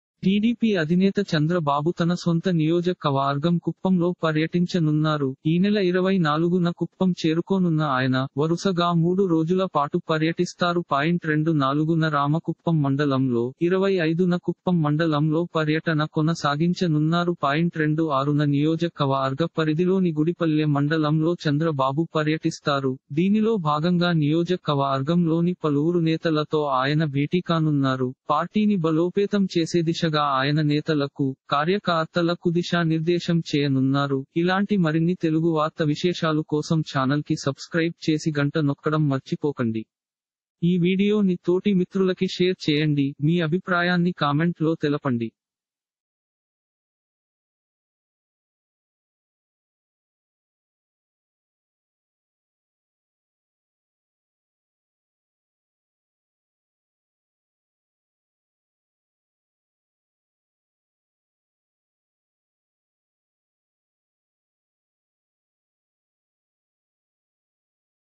TDP Chief Chandrababu Is Coming To Visit Kuppam - Telugu Chandrababu - కుప్పం ప‌ర్య‌ట‌న‌కు రానున్న
టీడీపీ అధినేత చంద్ర‌బాబు త‌న సొంత నియోజ‌క‌వ‌ర్గం కుప్పంలో ప‌ర్య‌టించ‌నున్నారు. ఈనెల 24న కుప్పం చేరుకోనున్న ఆయ‌న‌.. వ‌రుస‌గా మూడు రోజుల‌పాటు ప‌ర్య‌టిస్తారు. 24న రామ‌కుప్పం మండ‌లంలో, 25న కుప్పం మండ‌లంలో ప‌ర్య‌ట‌న కొన‌సాగించ‌నున్నారు. 26న నియోజ‌క‌వ‌ర్గ ప‌రిధిలోని గుడిప‌ల్లె మండ‌లంలో చంద్ర‌బాబు ప‌ర్య‌టిస్తారు. దీనిలో భాగంగా నియోజ‌క‌వ‌ర్గంలోని ప‌లువురు నేత‌ల‌తో ఆయ‌న భేటీకానున్నారు. పార్టీని బ‌లోపేతం చేసే దిశ‌గా ఆయ‌న నేత‌ల‌కు, కార్య‌క‌ర్త‌ల‌కు దిశా నిర్దేశం చేయ‌నున్నారు. - TDP Chief Chandrababu Is Coming To Visit Kuppam - Telugu Chandrababu $telugu-title:కుప్పం ప‌ర్య‌ట‌న‌కు రానున్న టీడీపీ అధినేత చంద్ర‌బాబు$ *pid:2122014* - Telugu News TeluguNews TeluguBreaking Telugu TeluguStop | Telugu news